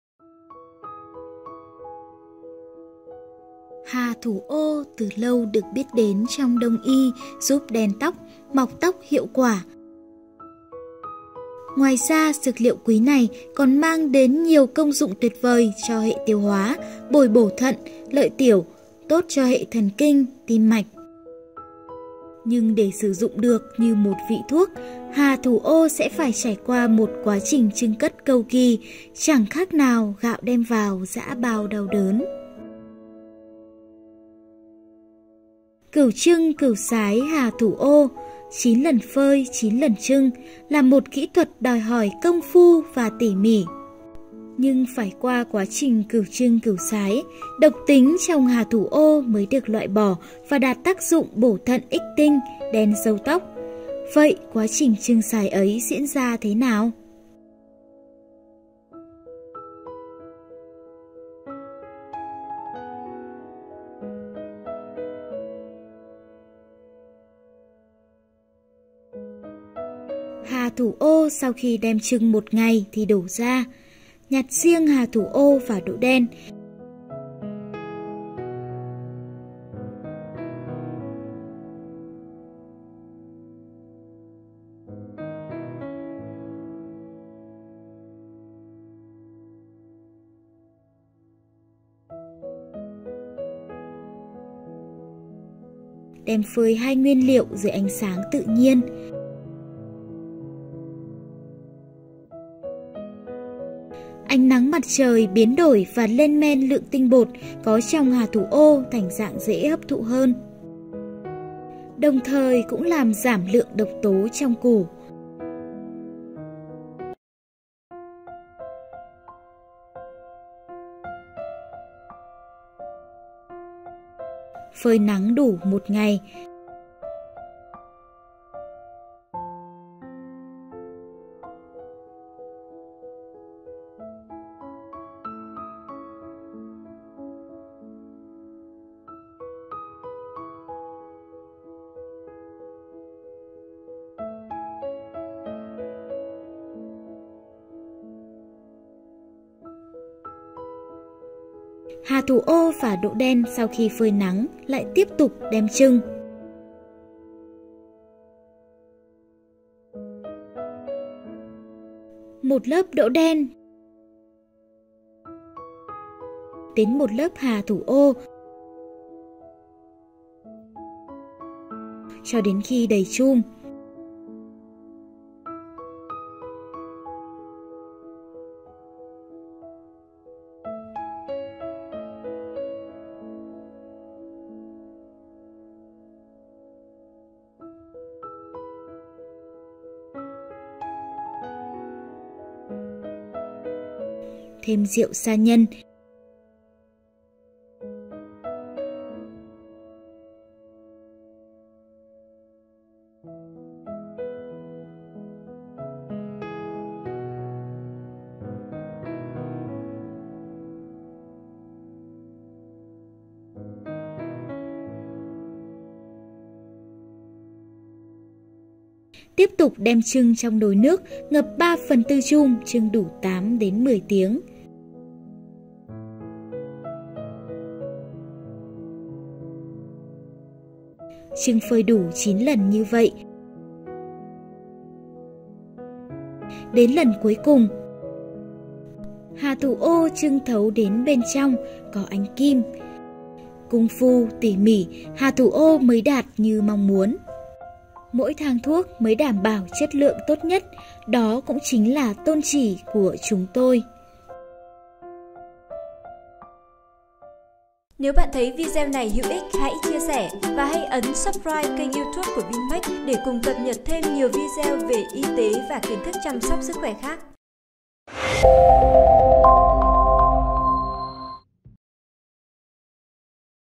Mãn nhãn xem quá trình 9 lần chưng - 9 lần phơi vị thuốc Hà Thủ Ô
Hà thủ ô từ lâu được biết đến trong đông y giúp đen tóc, mọc tóc hiệu quả.
Ngoài ra, dược liệu quý này còn mang đến nhiều công dụng tuyệt vời cho hệ tiêu hóa, bồi bổ thận, lợi tiểu, tốt cho hệ thần kinh, tim mạch...
Nhưng để sử dụng được như một vị thuốc, hà thủ ô sẽ phải trải qua một quá trình chưng sái, 9 lần chưng, 9 lần phơi nắng để loại bỏ độc tố và hội tụ các tinh chất chữa bệnh.
Mời bạn xem quá trình Trung tâm  Y học cổ truyền Vinmec Sao Phương Đông chưng sái vị thuốc này, nhằm đảm bảo công dụng cao nhất của vị thuốc.

Đăng ký Tư vấn sức khỏe từ xa tại